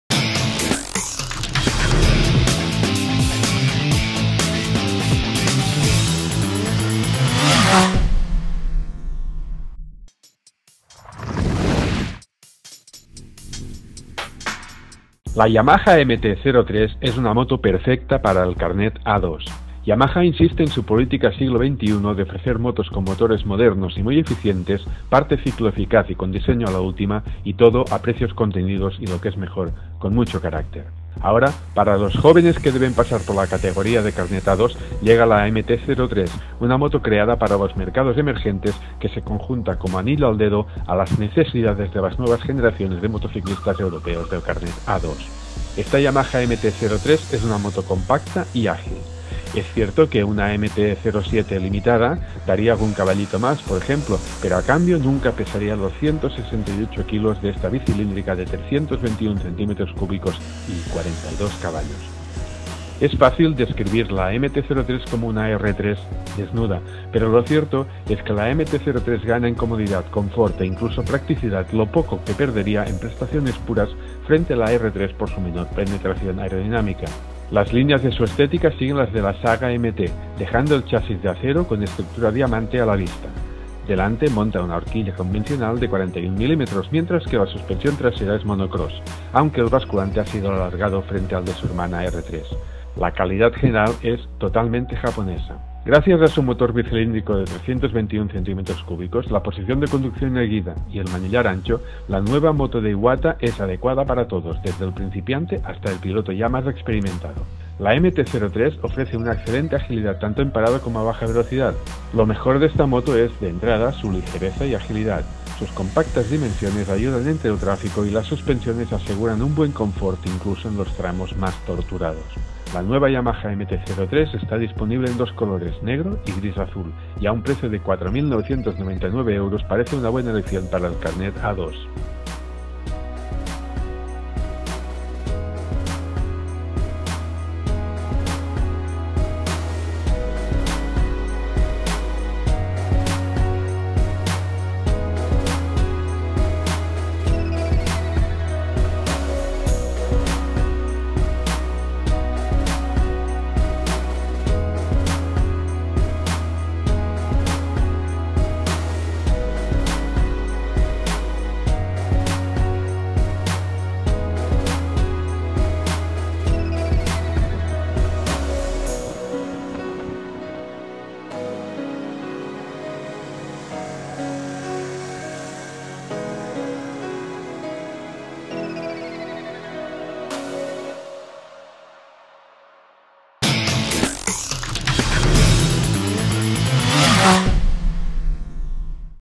Yamaha MT-03 - Prueba, opinión y detalles
La Yamaha MT-03 llena perfectamente el hueco en la familia MT entre la MT-125 y la MT-07. Una moto perfecta para la “condena” del A2... LEE LA PRUEBA COMPLETA EN EL SIGUIENTE ENLACE